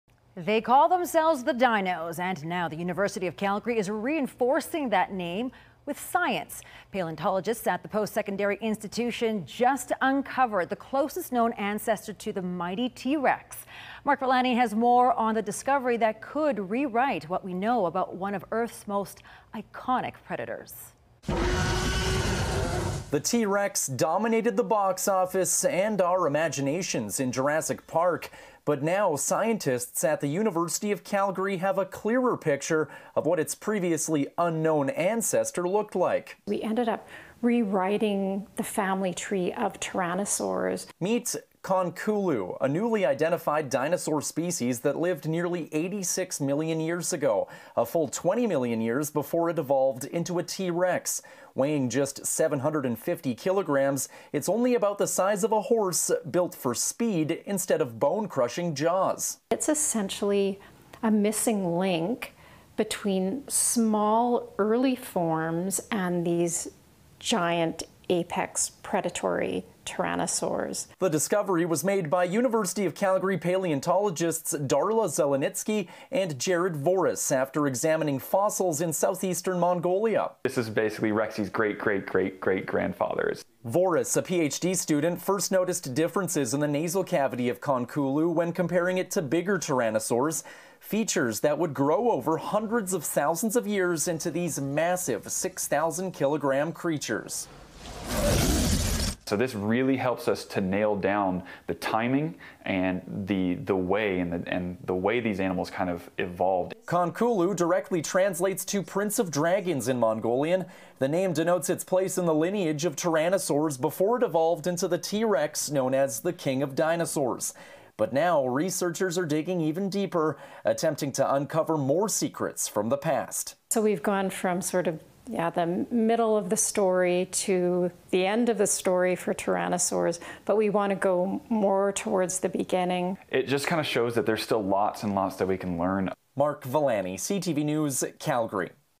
Discovery could rewrite what we know about the T-Rex
Paleontologists at the University of Calgary have just uncovered the closest-known ancestor to one of earth's most iconic predators: the mighty T-Rex.